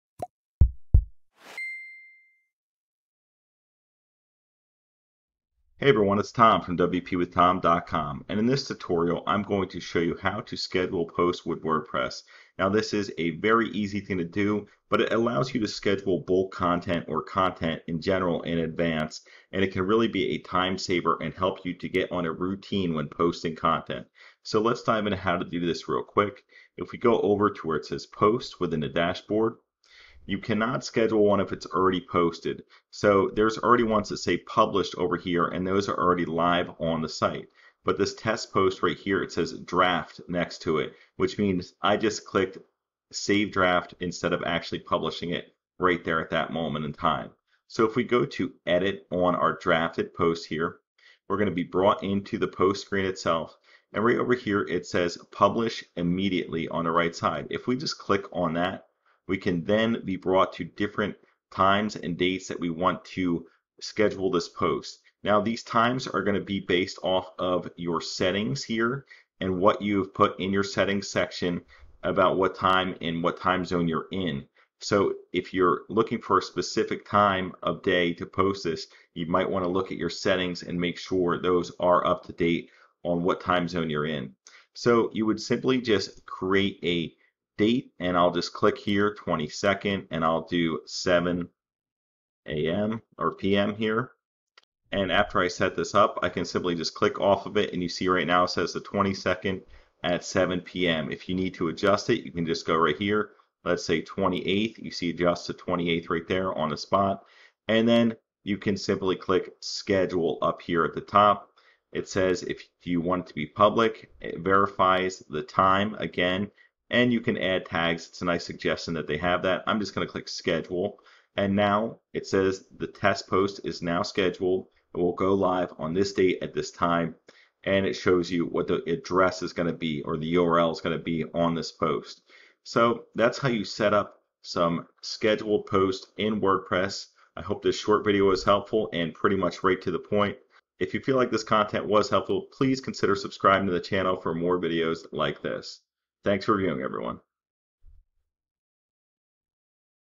How to Schedule WordPress Posts | Schedule Blog Posts
In this short video I cover how to schedule blog posts in WordPress. Scheduling blog posts with WordPress post scheduler option can be a huge time saver. On top of that, it only takes a few seconds to set up and it allows you to set up blog posts in bulk. I hope this video helps you learn how to set them up and save some time in the process.

Best Hosts:
Hostinger

WordPress Builders:
Elementor:

WordPress Themes:
GeneratePress:

SEO:
SEMRush: